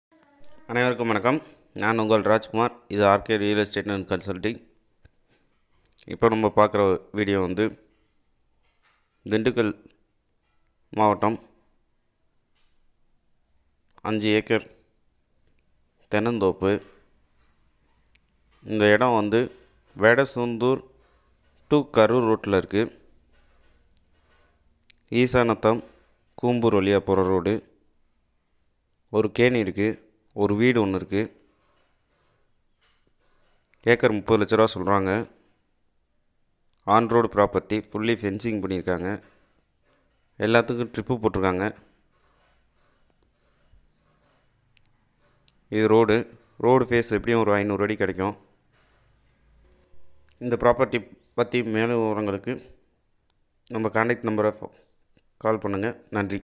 coconut farm sale in Dindigul 5 acre with house
coconut farm for sale in Dindigul to Karur road via Esanatham.kumbur...per acres 30 lak total price 1.50 cr negotiable price